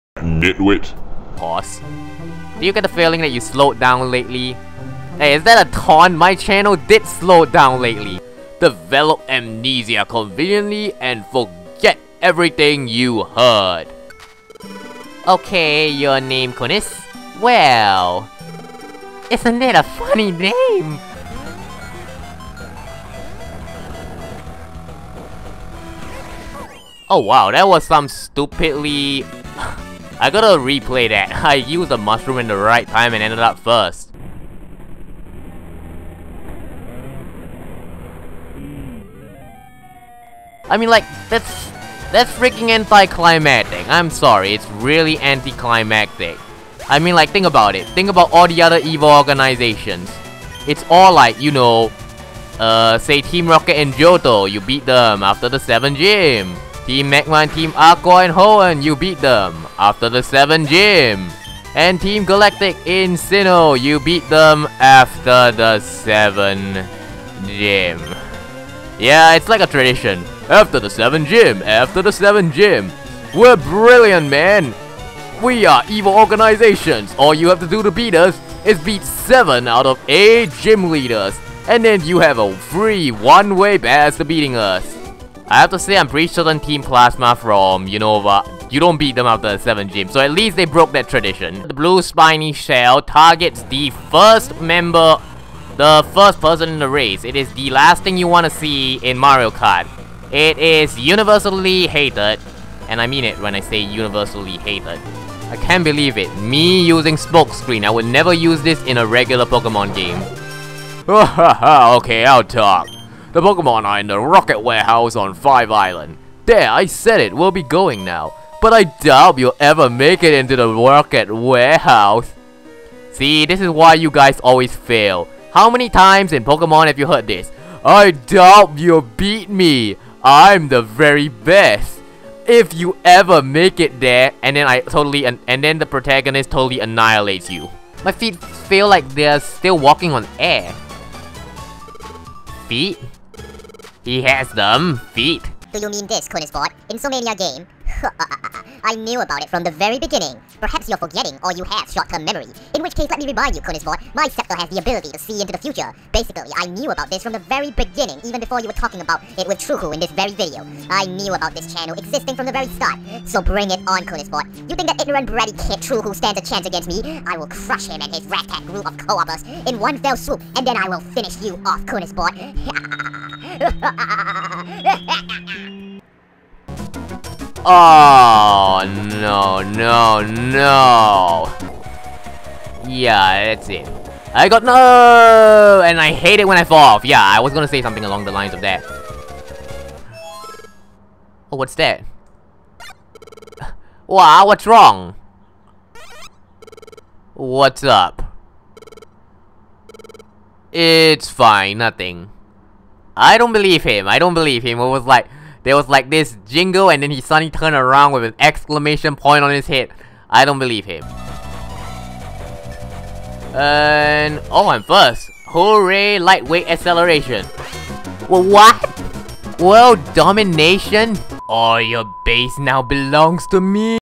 Best of April 2014 Moments Montage
The montage that showcases my (Konisbored) Anti Boredom channel's most noteworthy moments in April 2014. (which surprisingly is not as much as last time... xD)

Video thumbnail credits: Sprites/BGs courtesy of Google Images, made and edited by Microsoft Paint and GIMP.
Recording/Editing Software: Bandicam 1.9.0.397, Windows Movie Maker, Blue Snowball iCE (Mike)